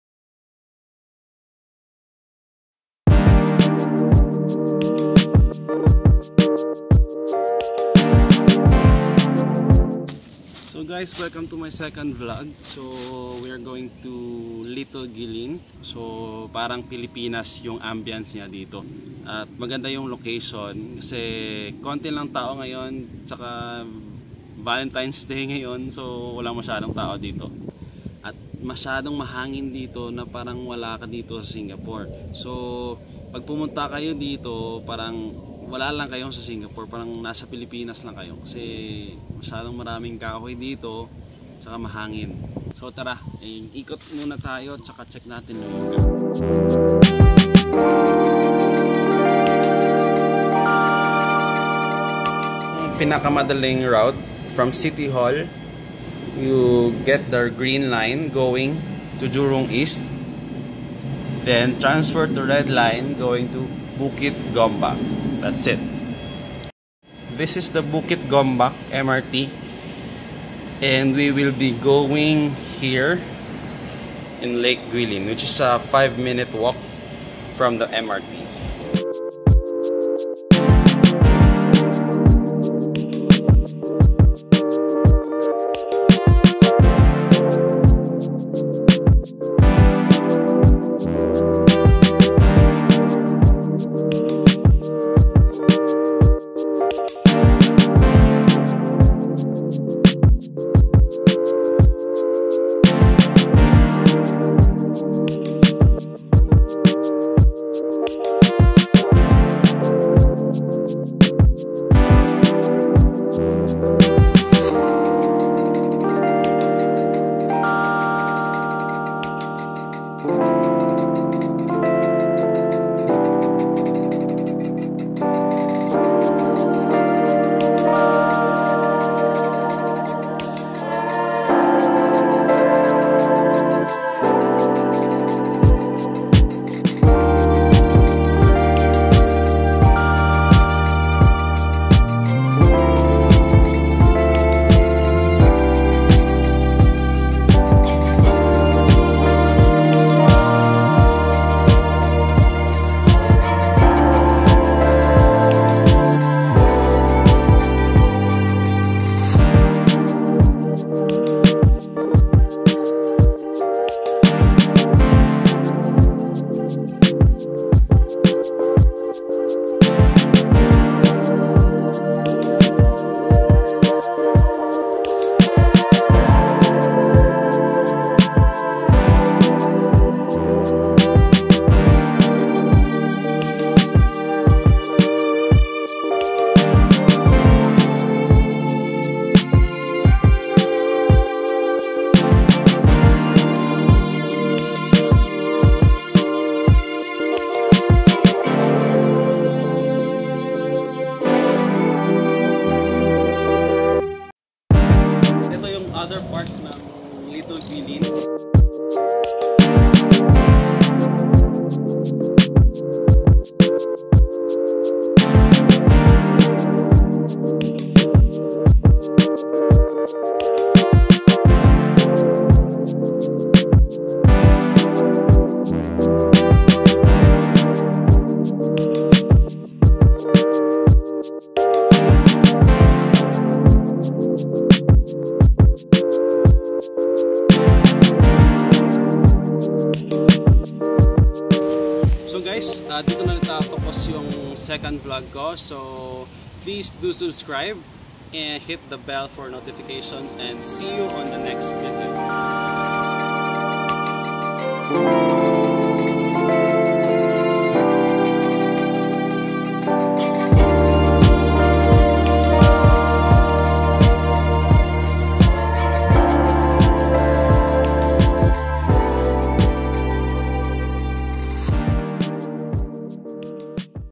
Singapore on a budget! Singapore Parks: Where is Little Guilin?
if you want to go to Singapore and just want to breathe some fresh air.
Little Guilin is the place to be.

just 2 stops away from Jurong east and you will be served with Philippine ambiance like place.